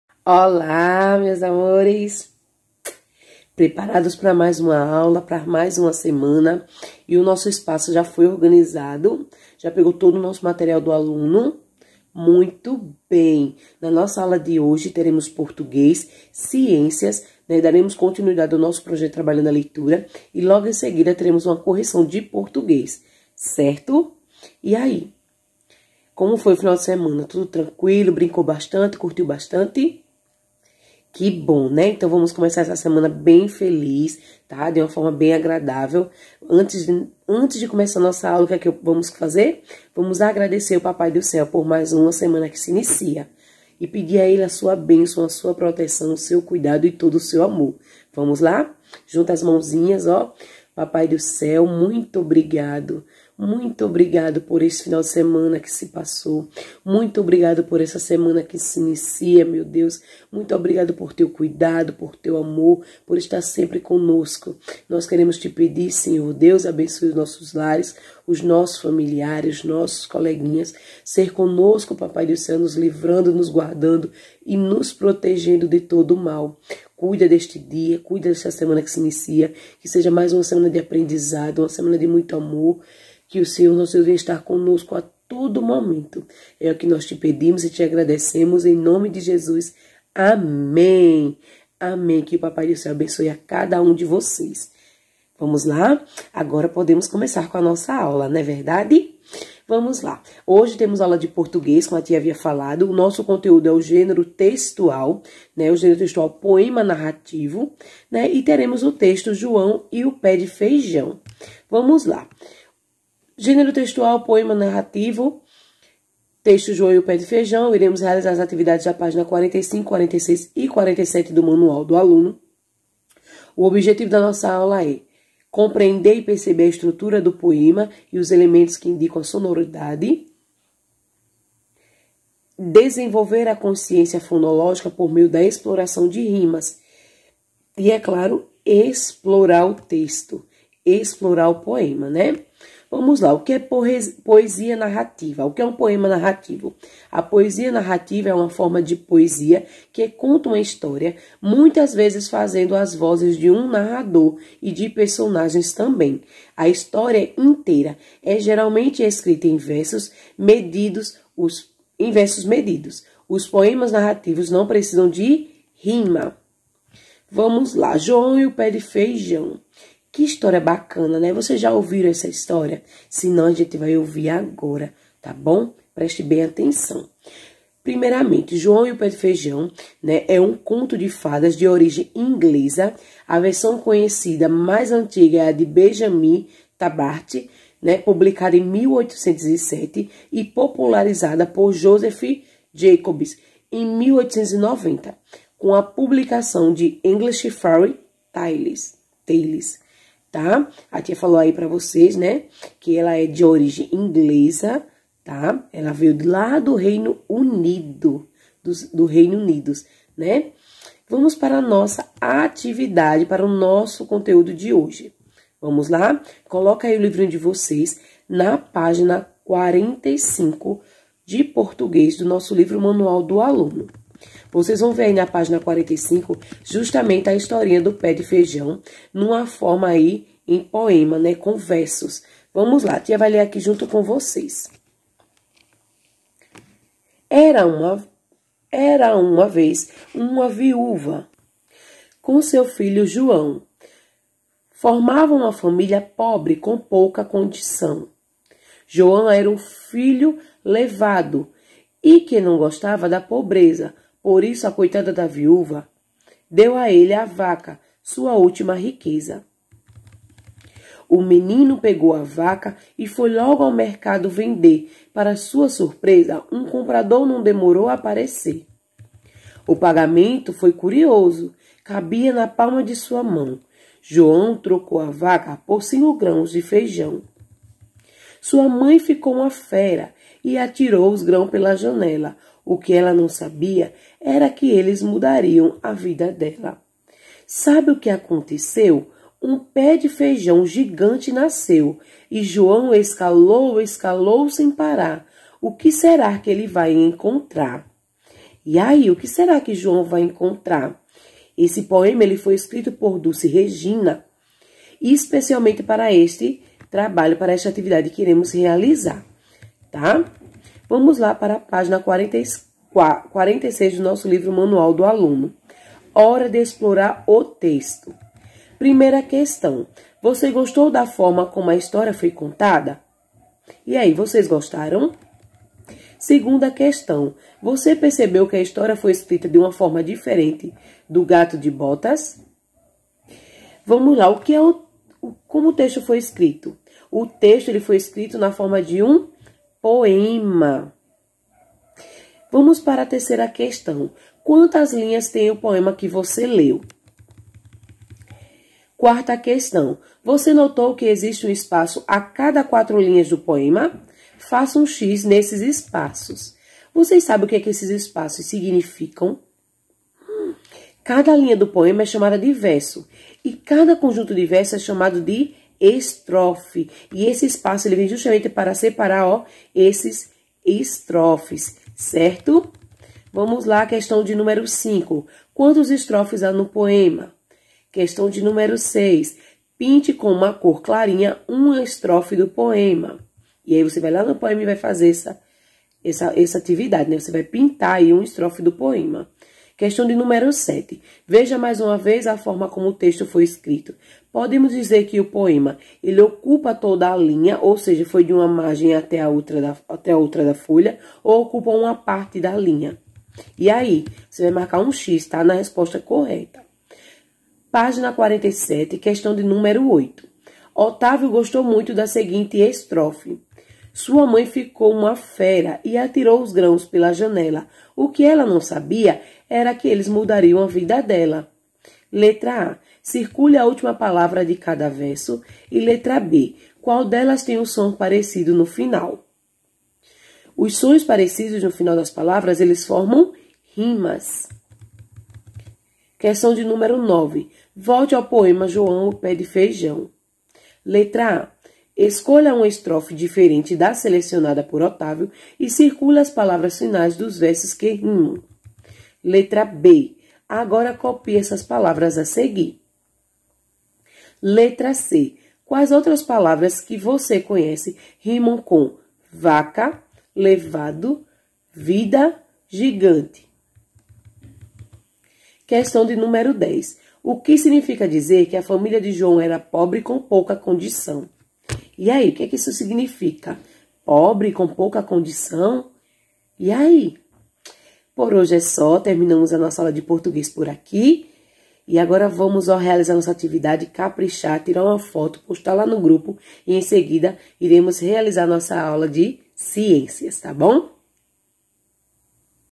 1° ano: Aula de Português - 27/07/2020
Aula de Português da turma do 1° ano com a tia Beth. Gênero textual: Poema narrativo - João e o pé de feijão. Páginas 45, 46 e 47. ❤